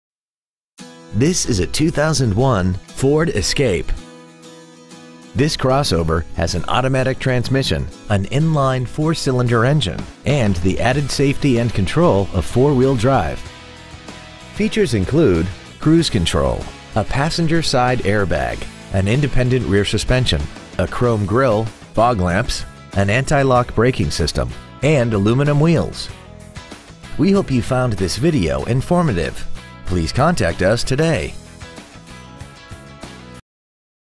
Pre-Owned 2001 Ford Escape Cedarville IL 61013
We have been honored to serve the Cedarville IL area , we promise that your experience at our dealership will exceed your expectations ! Year : 2001 Make : Ford Model : Escape Engine : 2.0L DOHC SEFI 16-Valve I4 Zetec Engine Trans . : Automatic Exterior : Red Miles : 146,285 Interior : Gray JH Barkau and Sons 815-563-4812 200 N. Stephenson Cedarville , IL 61013